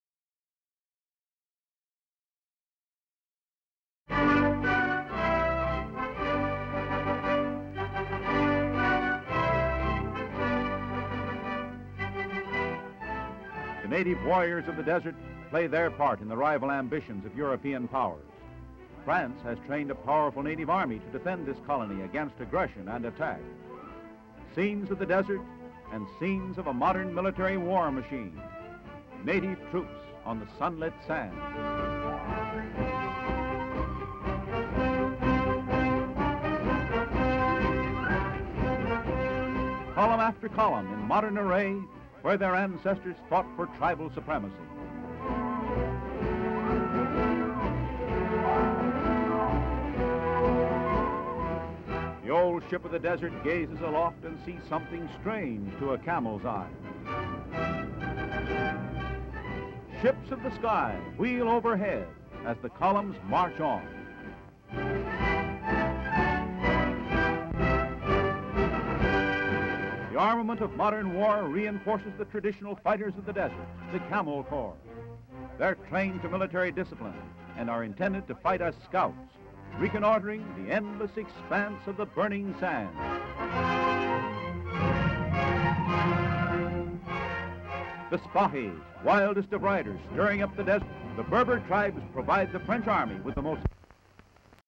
French colonial Army soldiers parade in desert in Kairouan, Tunisia. HD Stock Footage
French colonial Army soldiers parade in desert in Kairouan, Tunisia.

French colonial army in Kairouan, Tunisia. A map of Tunisia depicts the location of Kairouan. French Saphis on camels in a desert. French colonial troops march past a French officer. African Americans  march during the parade. Camels lift their heads to view planes flying overhead. Saphis ride camels and soldiers march. Tanks roll during the parade. Location: Kairouan Tunisia. Date:  1942.

57,000+ broadcast-quality historic clips for immediate download.